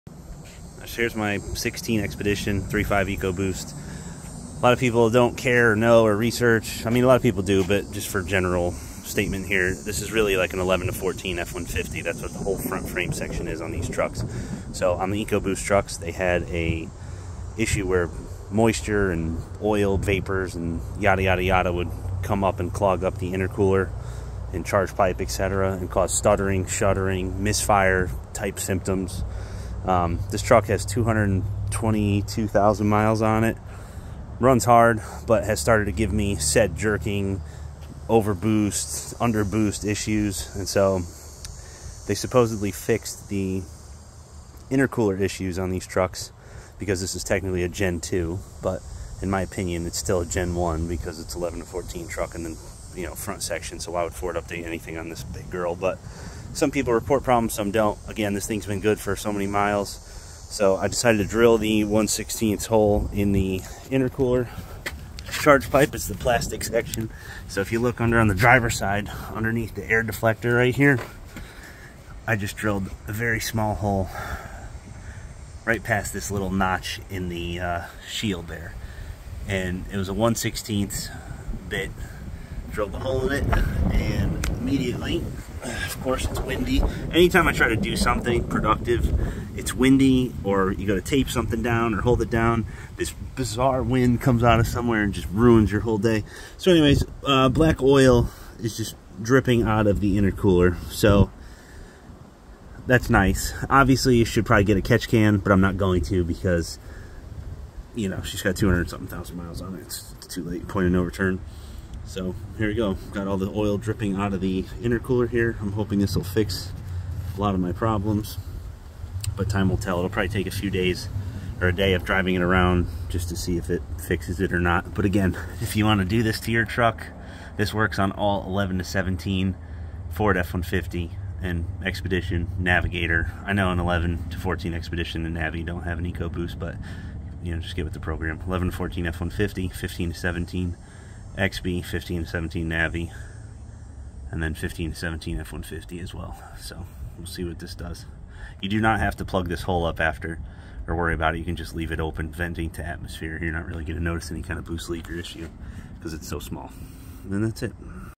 2015-17 Ford Expedition & Lincoln Navigator Intercooler Weep Hole Modification 3.5L Misfire fix
How to do the Weep Hole mod in the intercooler charge pipe for all Ecoboost F150, Expedition, and Navigator. Moisture and oil vapors build up in the intercooler and can cause shuddering, misfires, surging etc. So drill a 1/8 hole in the pipe and let it drain. I did not plug mine back up after drilling the hole, can leave it open to atmosphere. Will not affect performance or mileage etc.